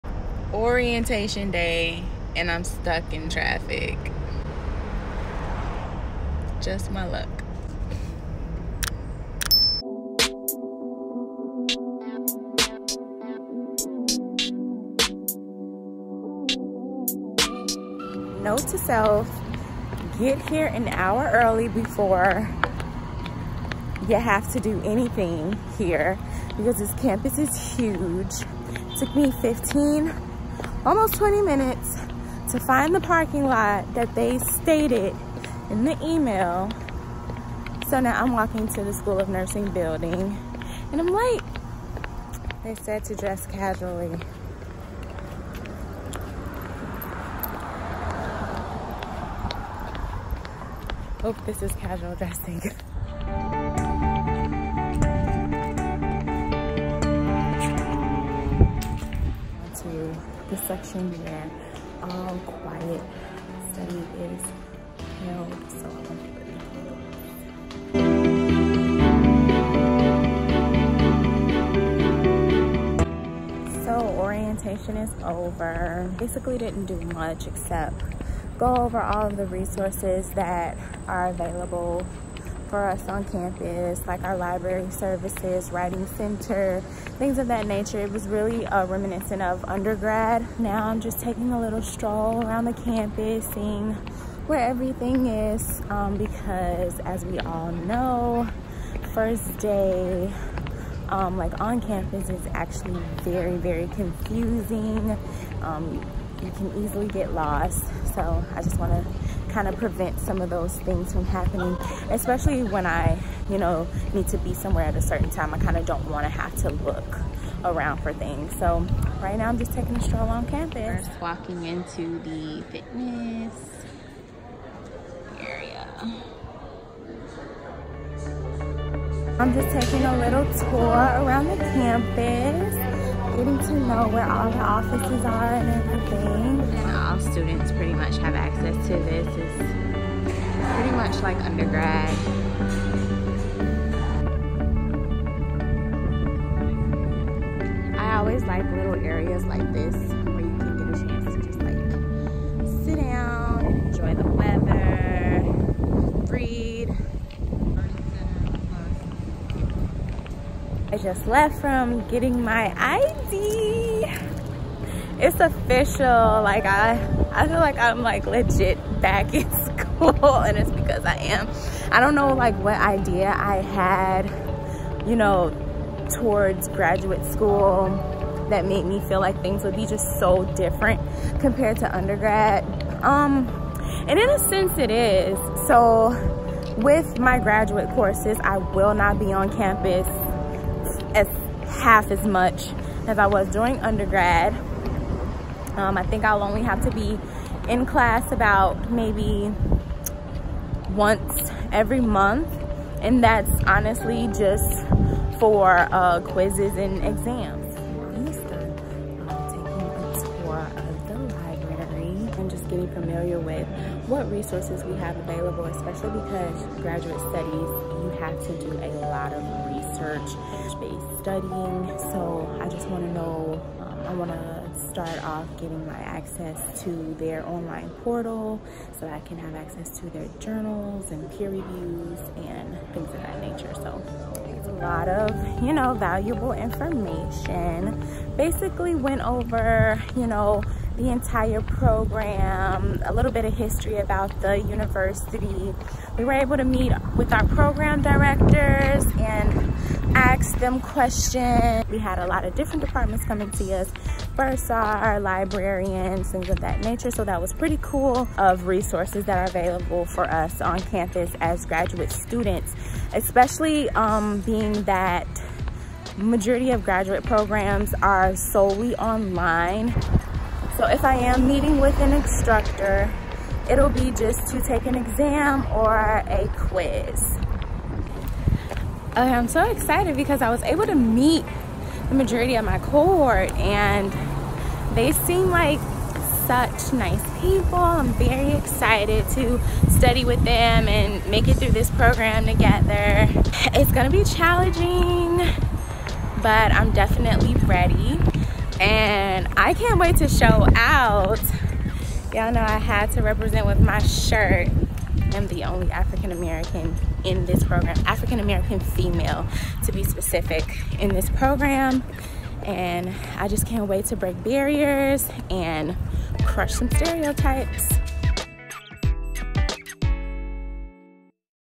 MSN NURSE PRACTITIONER ORIENTATION!
Come with me on my first official day beginning my journey towards becoming an MSN-Acute Care NP!

Unfortunately I wasn't able to record during the actual orientation due to copyrights of the power point presentations. However, if you have any questions specifically about what went on during the presentations let me know down in the comments and I'll make sure to answer!